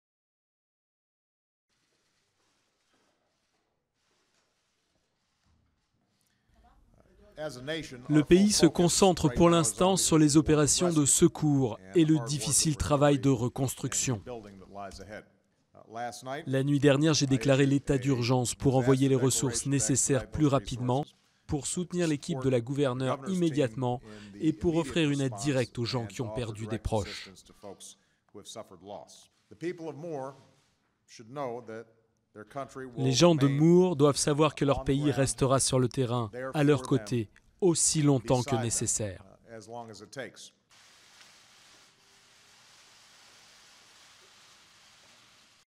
Tornade d'Oklahoma City: Obama promet la solidarité du pays
Le président Barack Obama a promis mardi que les Etats-Unis seraient solidaires des victimes et sinistrés d'une puissante tornade qui a dévasté la veille une ville dans la banlieue d'Oklahoma City (sud) et fait au moins 24 morts. Durée: 00:45